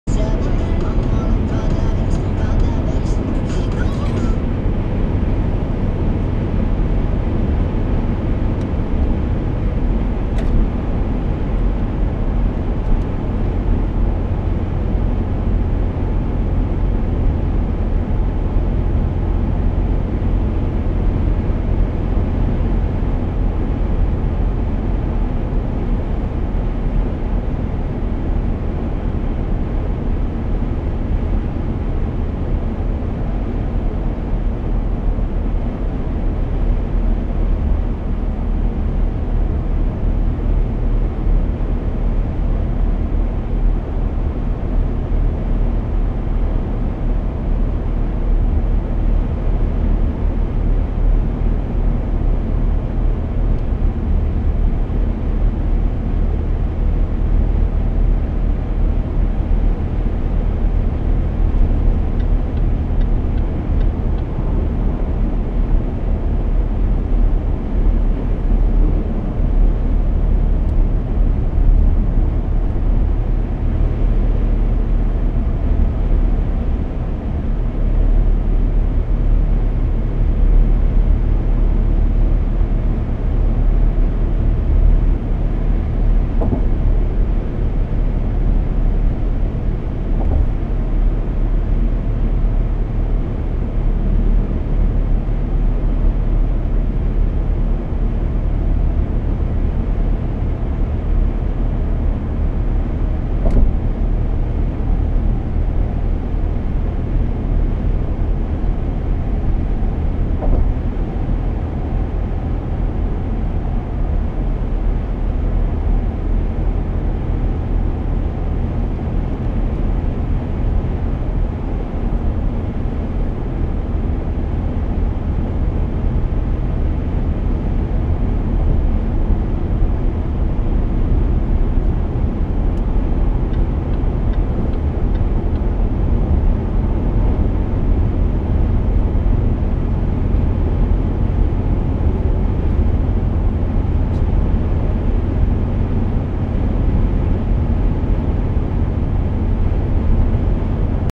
Autobahndreieck Havelland A10 West - A24 AUTOBAHN
Nach Umbau Fahrt durchs Neue Autobahndreieck.